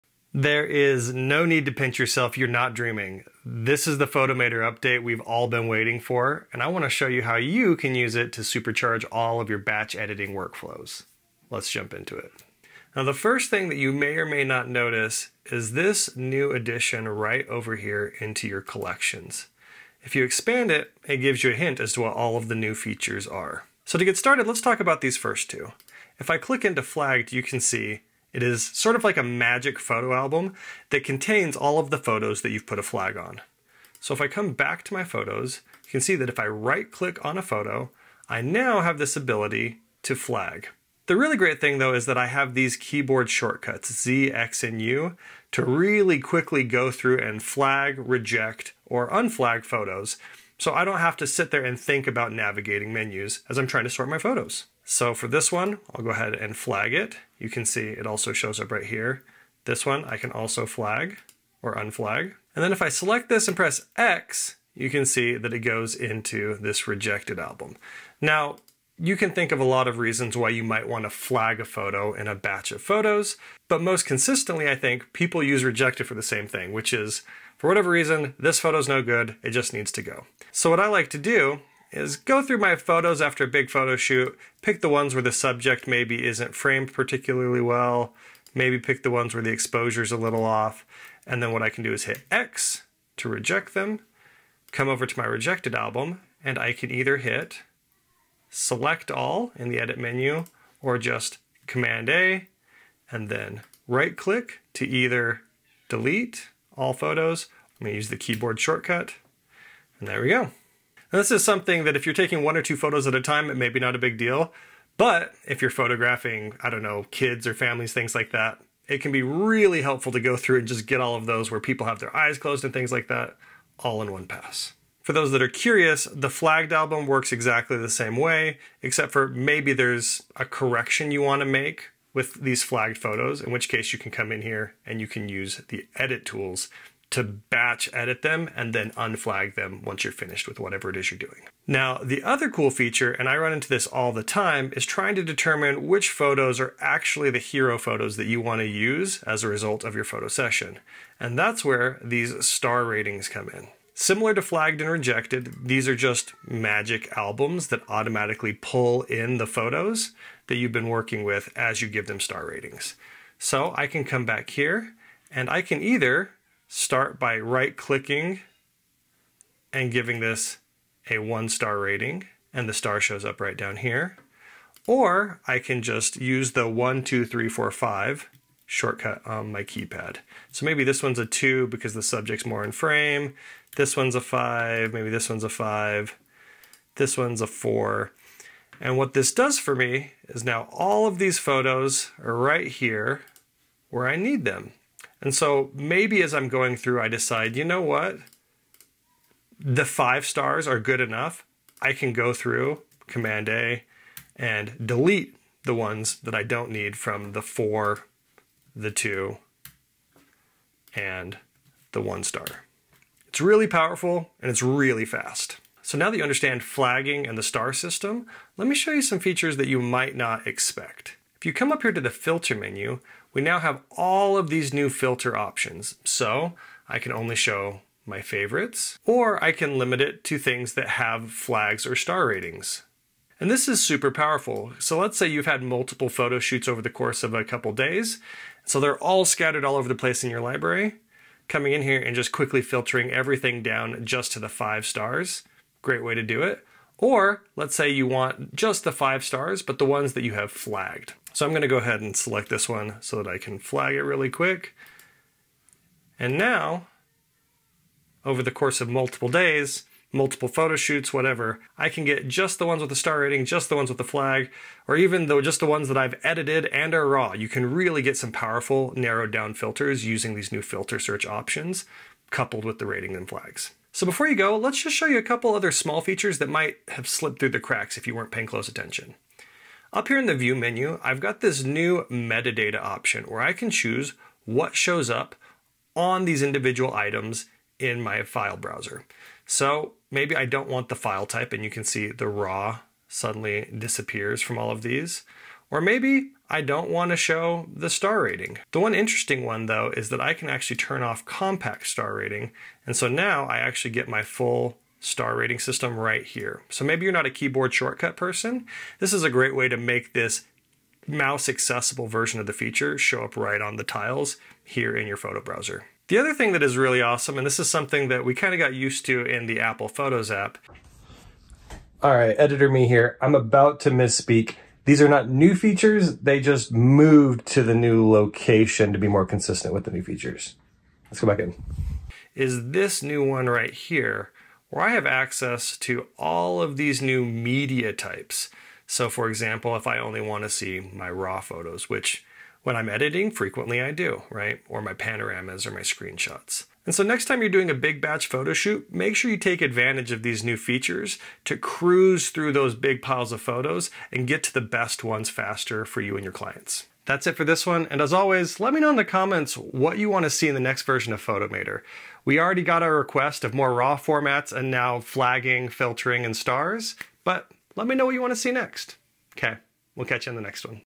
Photomator 3.4 is the update you've waited for
Photographers leaving Adobe Lightroom to Photomator will love these new features. Star ratings, flags, rejecting, and a few extra hidden gems. These tips and tricks will super power your professional photo editing game.